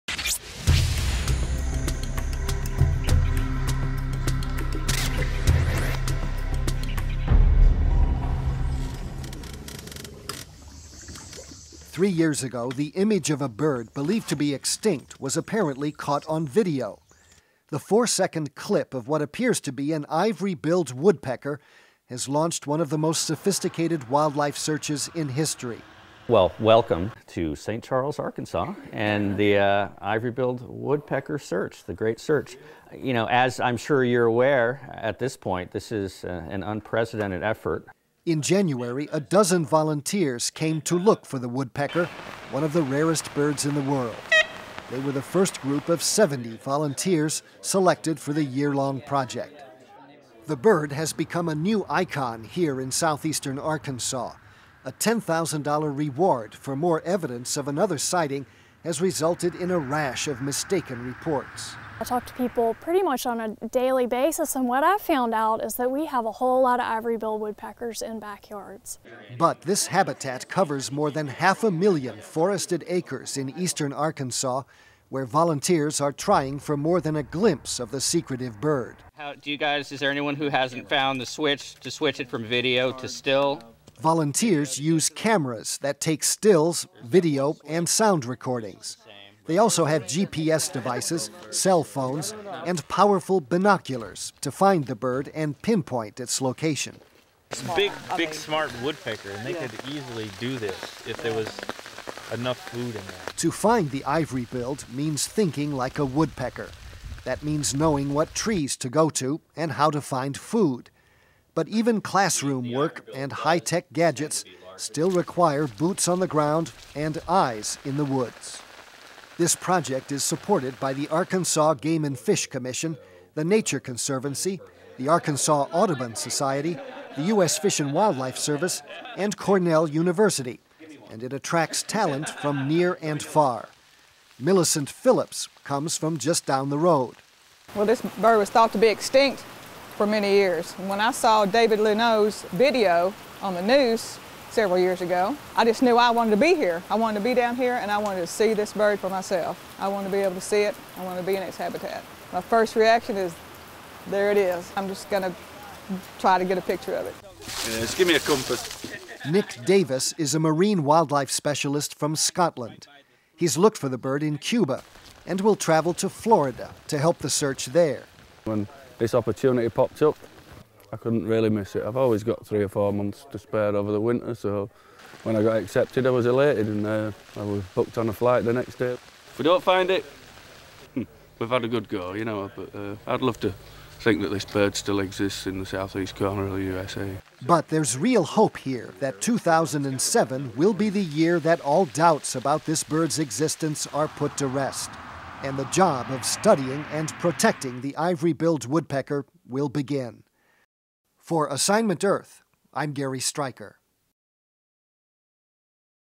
Ivory Billed Woodpecker
Three years after an apparent sighting in an Arkansas swamp of a bird believed extinct for decades, hopeful scientists and volunteers are still searching for it.